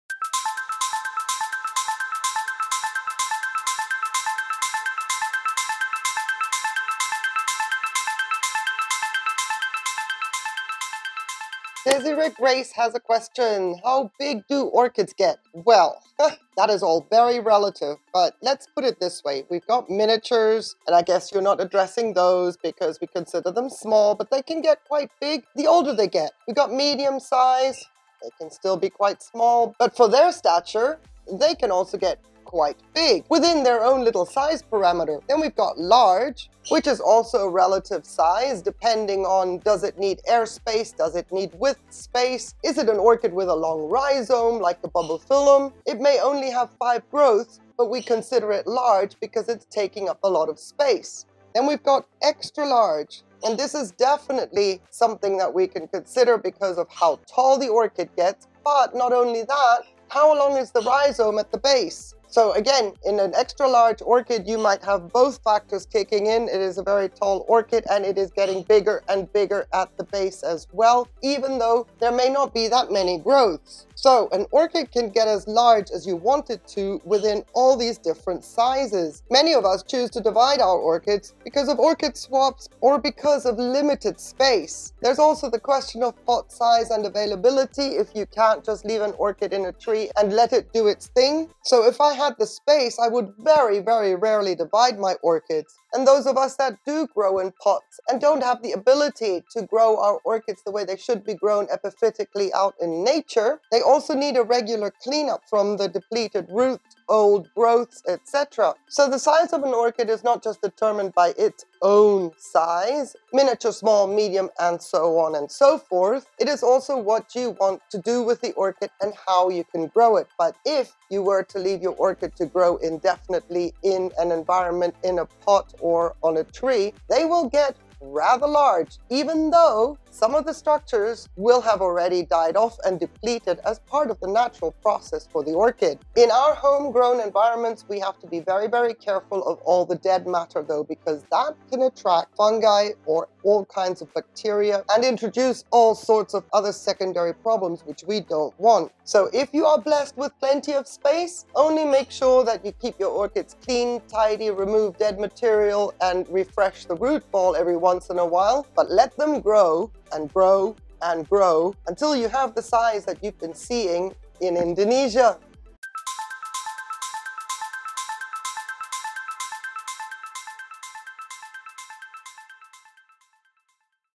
How big can orchids get? | How much space do you have? 👍🏼👍🏼
Specimen size orchids are the goal of every orchid grower! We buy them when they are divisions, or seedlings and want to have them grow to their maximum potential, and bloom from as many growing points as possible. But... is that feasible? It is, IF you have unlimited space and the right conditions for the orchids you are growing! Desire Grace, thank you for your question, here is your Ninja Clip
My environment:
Mediterranean climate, hot summers / mild winters
45% - 85% humidity, with regular airflow
Thumbnail / Outro Image:   Bodum.westernscandinavia.org
Orchid growing supplies:
               Seramis, Akadama
Water: Reverse Osmosis
Insecticidal Soap
General PPM: 300 Summer, or in active growth
                          150 Winter, RO Water and KelpMax only